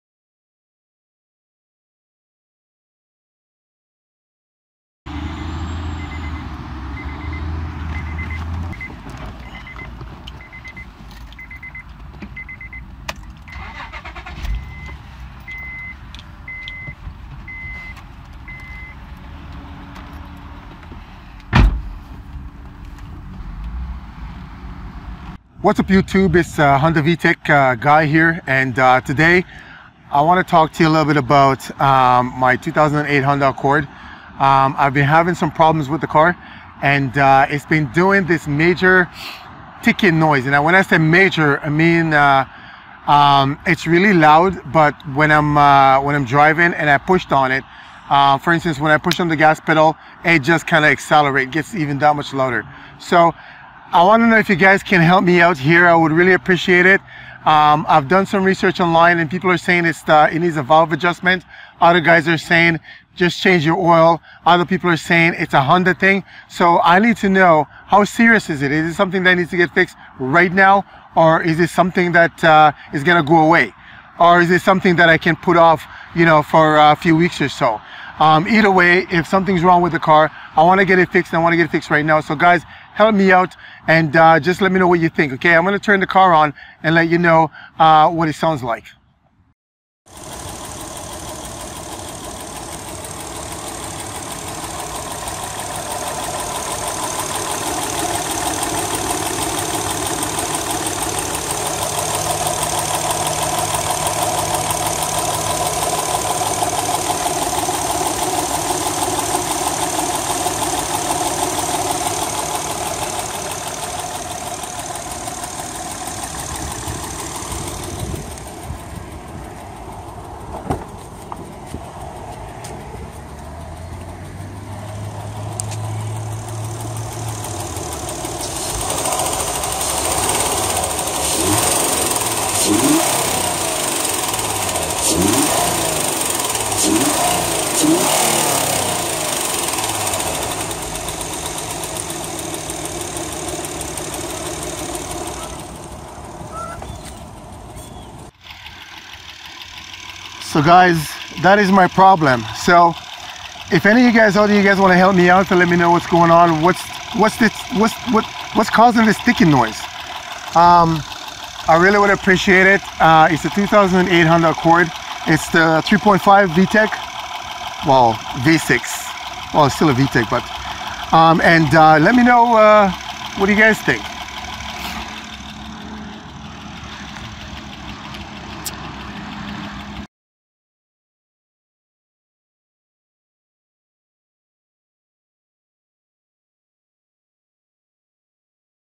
8th Gen Honda Accord ''Engine Making Loud Ticking Noise''
Hey guy's my 2008 Honda Accord Coupe has been making this very loud ticking noise and I have no idea what's causing it. If you can help me out with this problem I'd appreciate it greatly... Thank you!